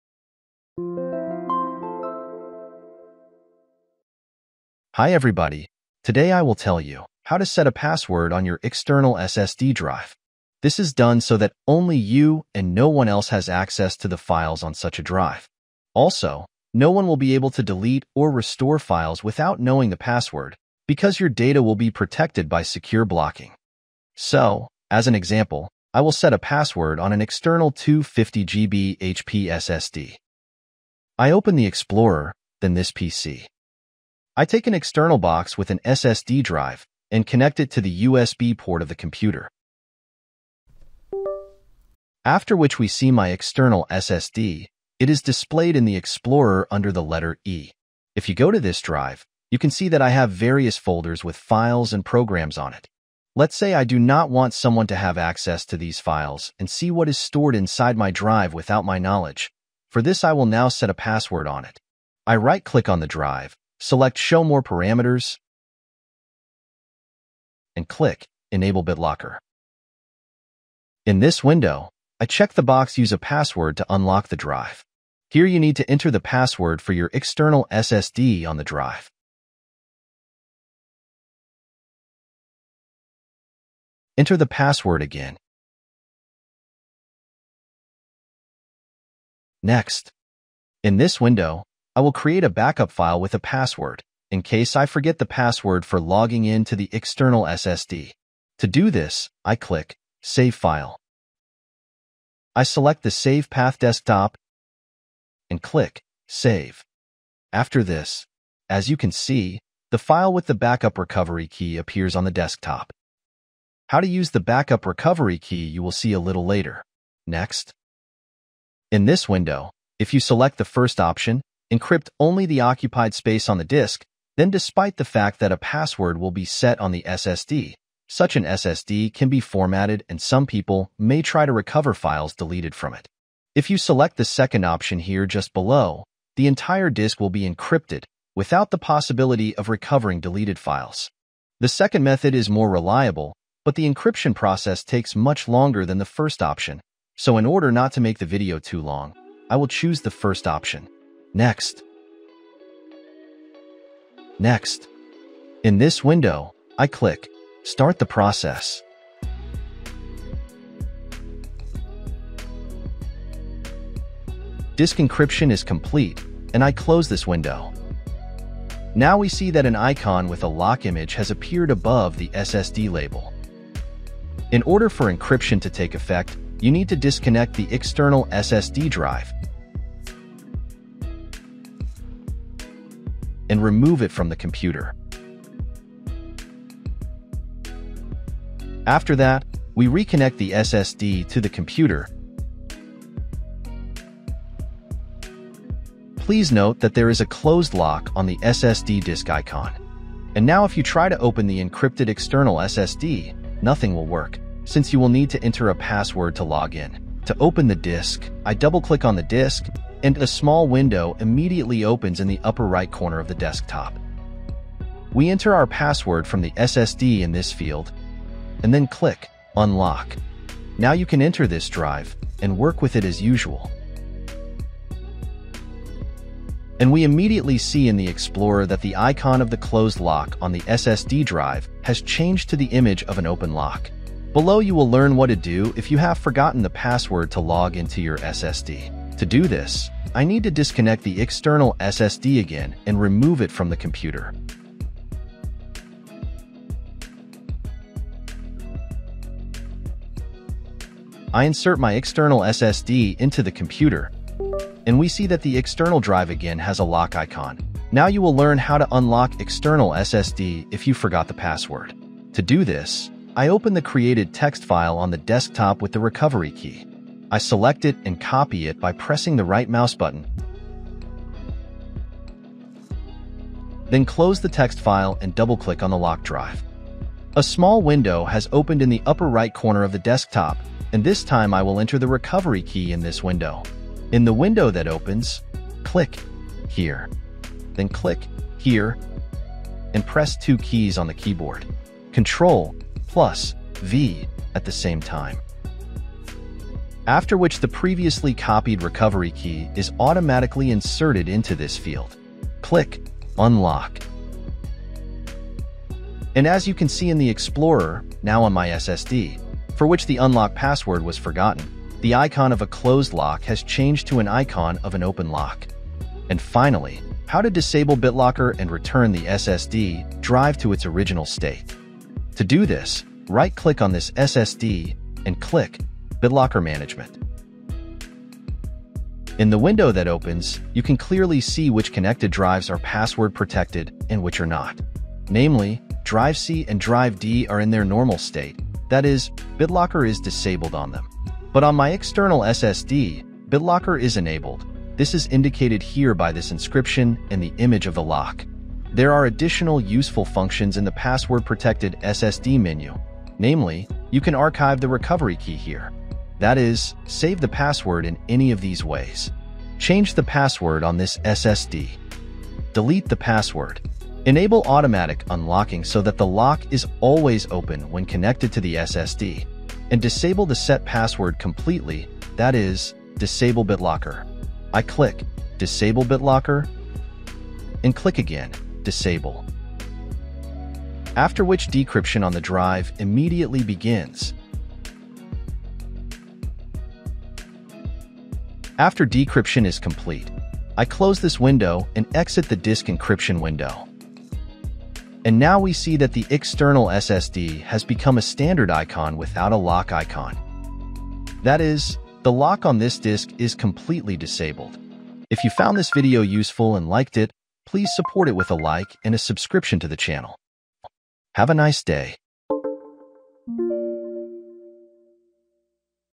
🔒How to Password Protect External SSD in Windows ➡️Without Third-Party Software💯
Hi, if you are interested in the most convenient way to work with your secret data in Windows 11 or Windows 10 without third-party programs, and so that your loved ones or strangers who have access to your external drive do not see this data, then watch this video, and today you will learn how simple and easy it is to do this using the Bitlocker program built into Windows 11 and Windows 10.
This method works for the following types of external drives - HDD, SSD and SSD NMVe
   👉How to encrypt a folder, disk, USB on Windows 11, 10 Home without Bitlocker:
🕑Timestamps
0:00 Intro
1:00 Enabling Bitlocker and setting it up.
2:36 Enabling encryption.
3:27 How to unlock a drive with a password.
4:18 How to unlock a drive if you forgot the password.
5:18 How to completely disable Bitlocker